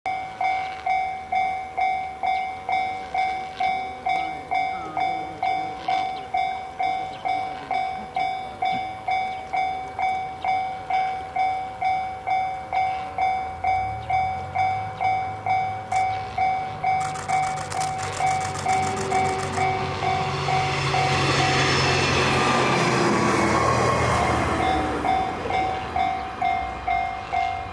普通９４３D伯備線榎踏切通過(2024.5.18)
令和６年５月１８日、伯備線榎踏切付近を通過する、キハ１２１－８単行、生山発米子行き、普通９４３Dの様子。